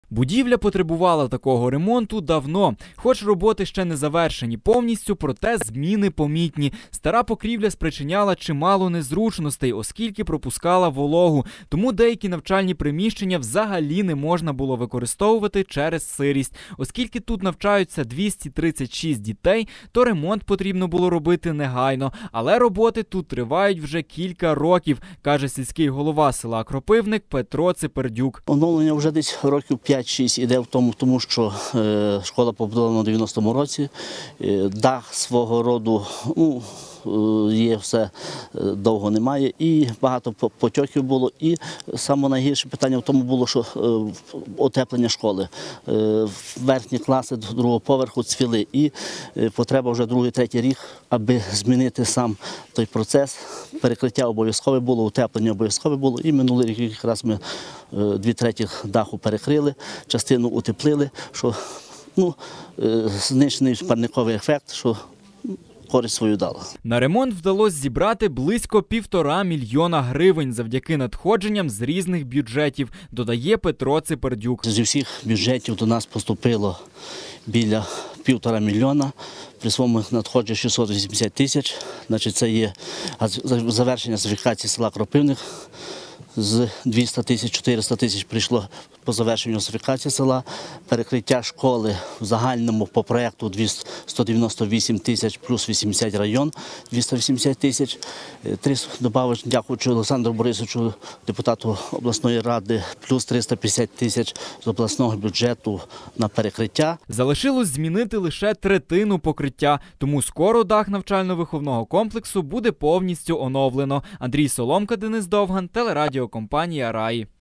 У с. Кропивник на Долинщині здійснили оновлене покриття навчально-виховного комплексу
У с. Кропивник на Долинщині здійснили капітальний ремонт даху тамтешнього навчально-виховного комплексу. Деталі у сюжеті